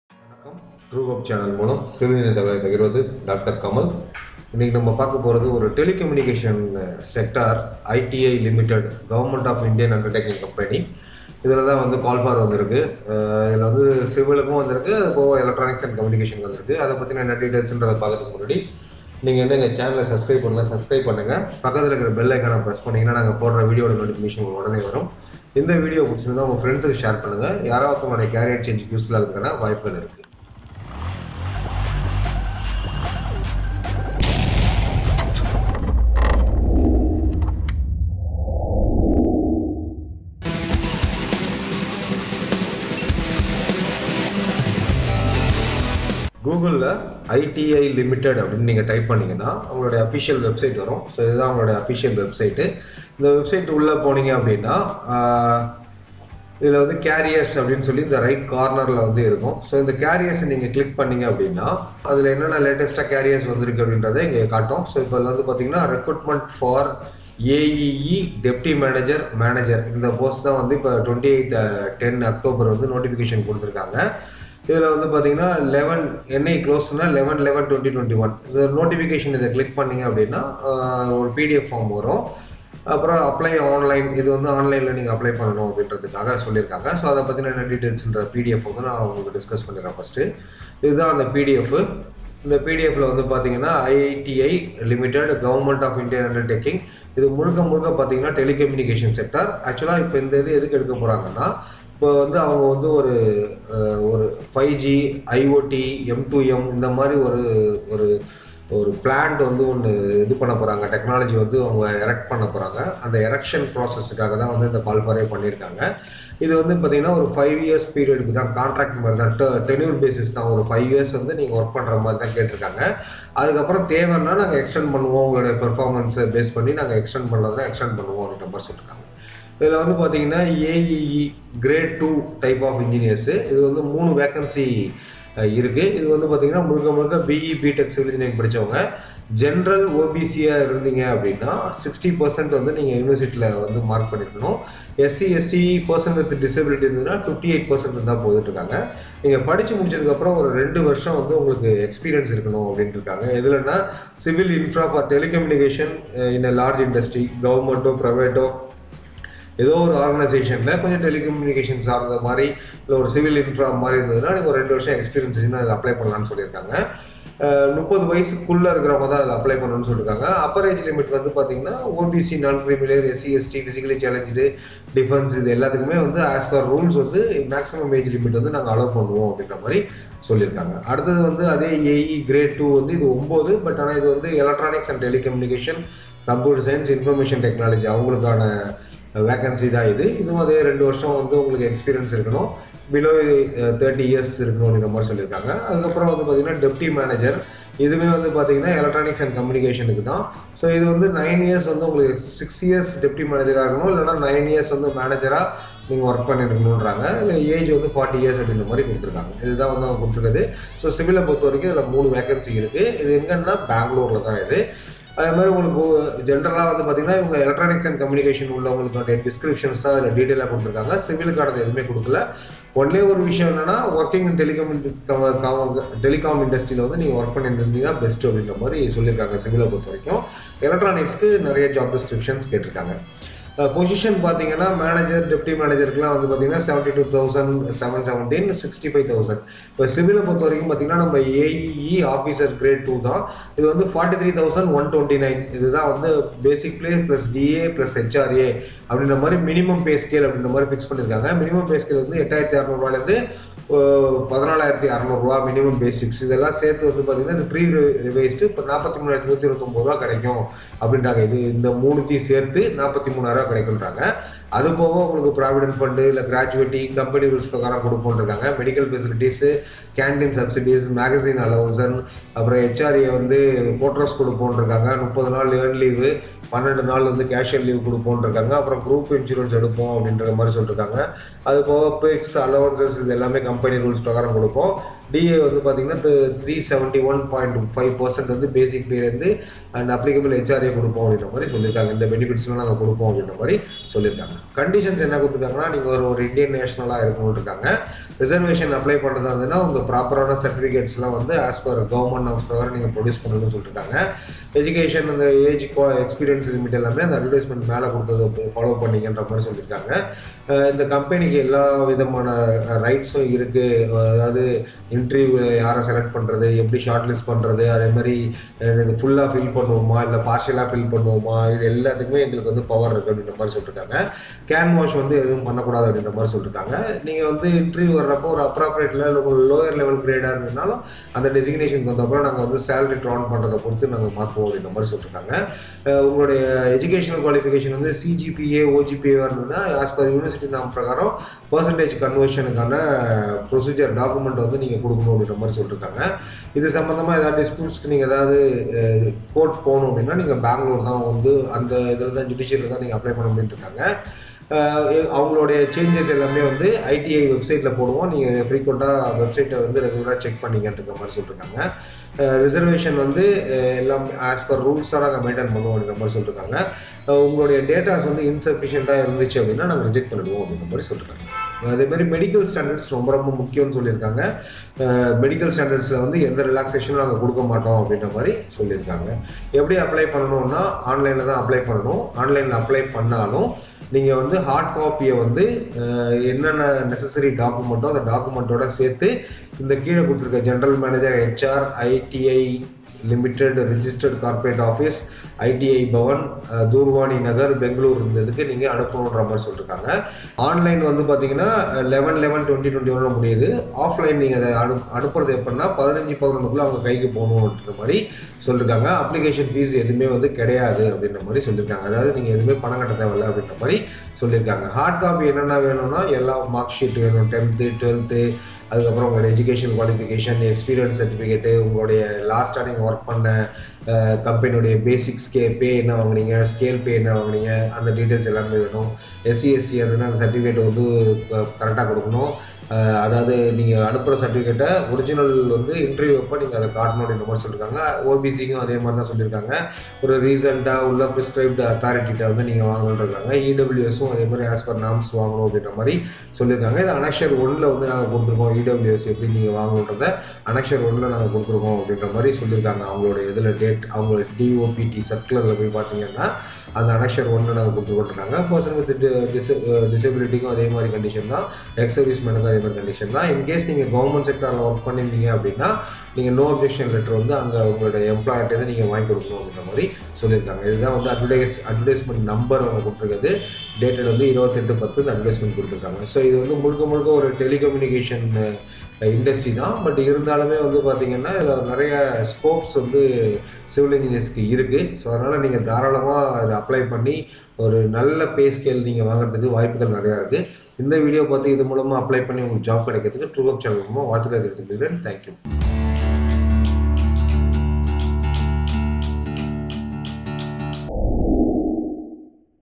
ITI Ltd recruitment| Civil Engineers Job in india|latest 2021 civil jobs|civil job in telecom
Hi good morning to all
This video is about the recruitment of Assistant Executive Engineer (Civil), Deputy Manager Engineer (ECl) for civil engineers and EC,CSE, in ITI ltd bengaluru.
It is a good opportunity for civil engineers to get the latest engineering jobs to enhance their career in a reputed institution.
This ITI has invited applications for civil engineers to join their department during the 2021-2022 engineer list.
This video is discussing the way you can see the advertisement and how to apply for the post called by the company. The discussed vacancy is based on openings given in the websites.